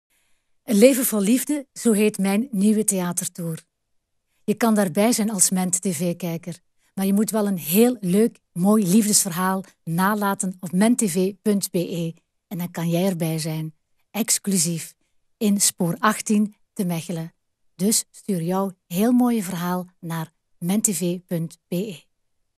Dana Winner Tickets te winnen exclusieve try-out Dana Winner 'Leven vol liefde'
Tickets te winnen exclusieve try-out Dana Winner 'Leven vol liefde' in Mechelen, spoor 18